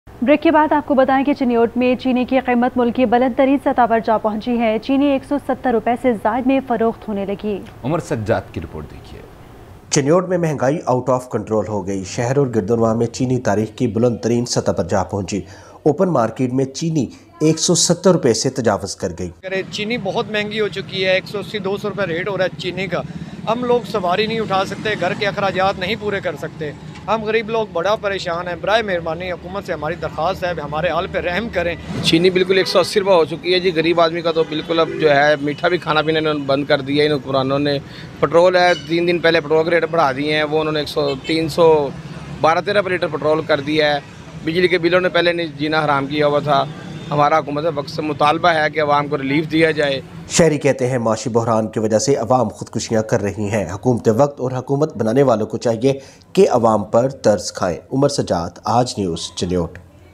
Sugar Prices reached the highest level in the country - Aaj News
Sugar Prices reached the highest level in the country - 04 September, 2023 - Aaj News

Spot Light With Munizae Jahangir:
shorturl.at/gwFU6